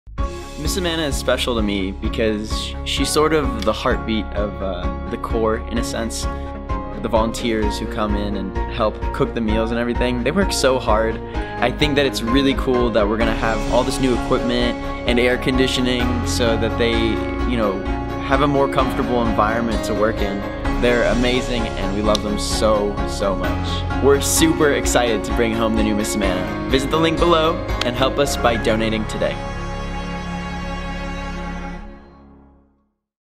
10 Days Left!
Not only will the new food truck benefit our members - it's going to make things easier for our volunteers too! We're only 10 days away from bringing Miss Amana home to Santa Clara. Can you help us reach our $100K goal?